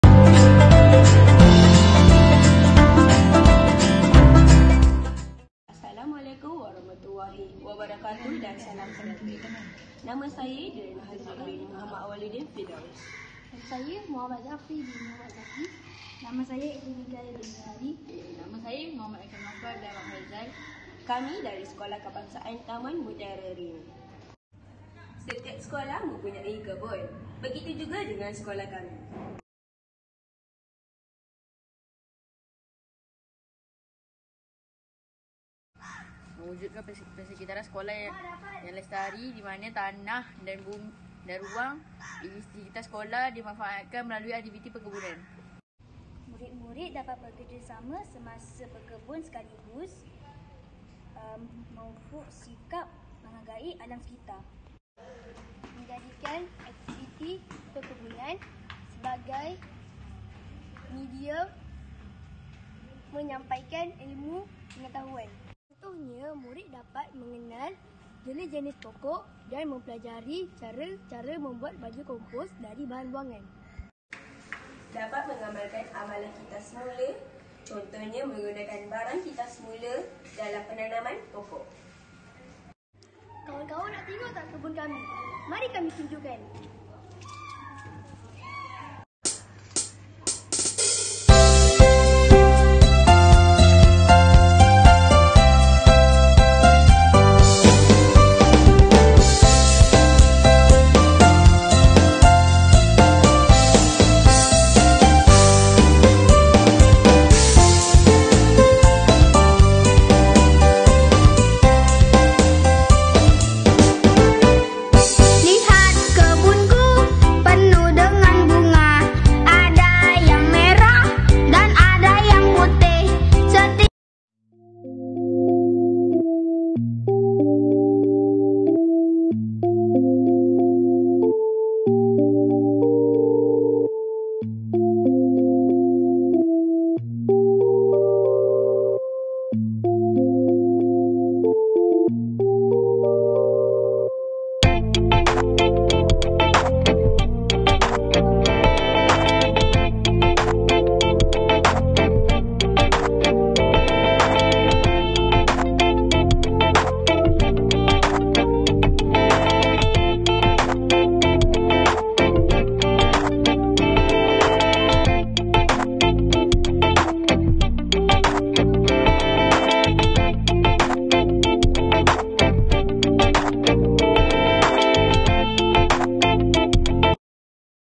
KEBUN SKTMR- Memanfaatkan tanah dan ruang
Kumpulan 1

Darian Hazreeq
Ikram Naufal
Muhammad Zafri
Adrean Mikael

Description

Kebun Sekolah telah bermula pada tahun 2019 dan 2020 dengan melaksanakan Projek Fertigasi. Berbekalkan semangat yang tinggi, pelbagai usaha yang dilakukan untuk memastikan tanah dan ruang di SKTMR dimanfaatkan dengan sebaiknya. Akhirnya, kebun sekolah terus berkembang sehingga berjaya mewujudkan Laman Hijau Mutiara, Laman Mutiara D’Herbs, Laman Bunga Raya, Laman Kompos dan Laman Tayar Mutiara.